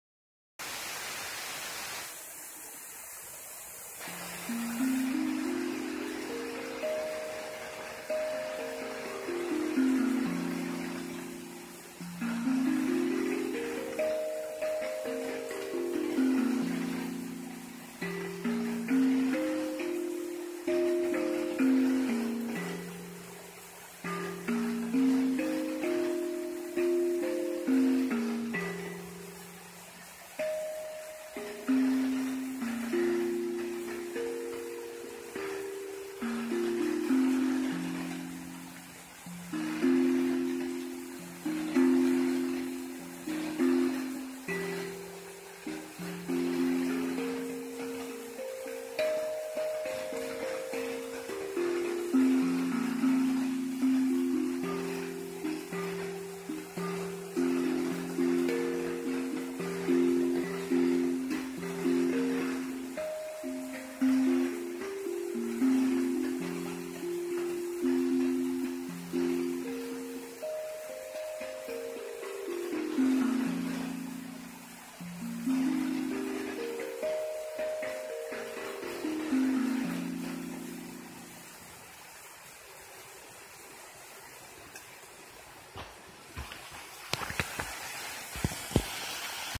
Satori Drum and waterfall duet
I took this G Major Pentatonic Satori Drum to this magical waterfall and played a lil duet. The energy here is so amazing. Look for more videos from this place soon!